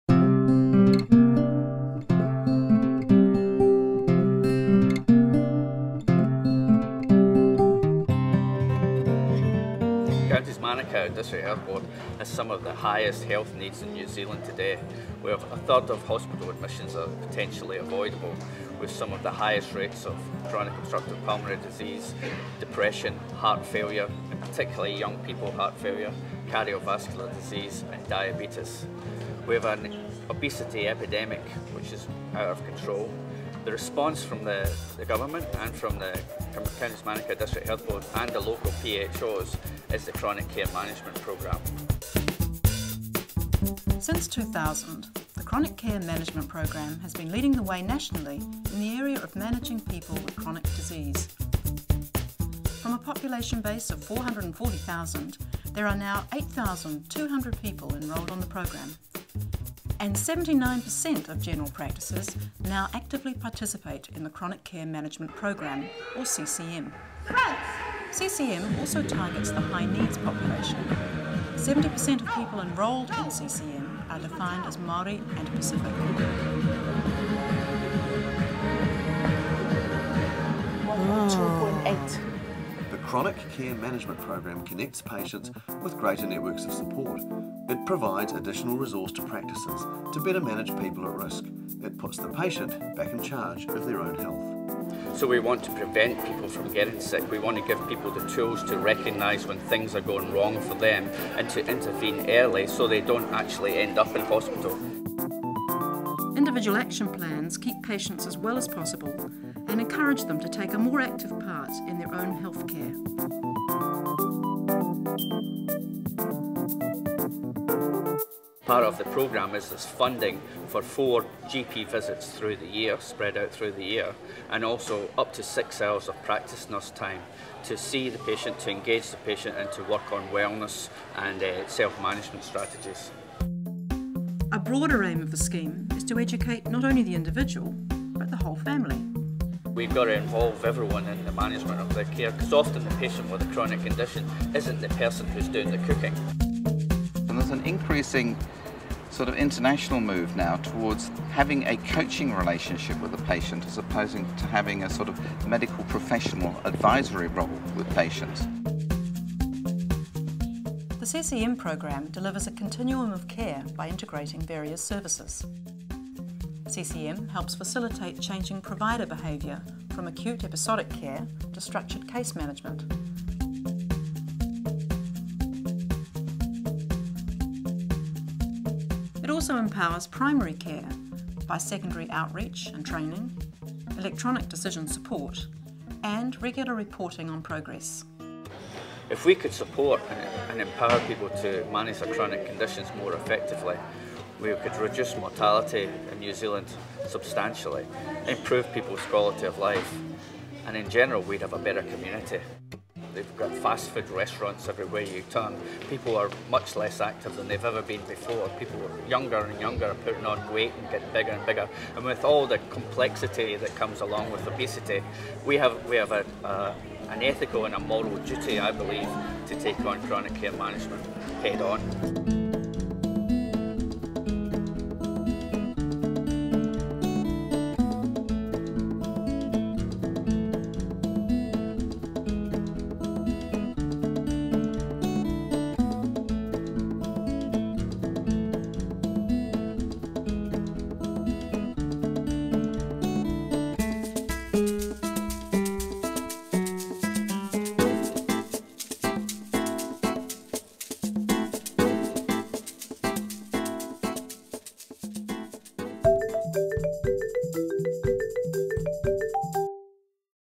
CMDHB Chronic Care Management - Overview
Chronic Care Management programme being run by Counties Manukau District Health Board explained in brief here in an Overview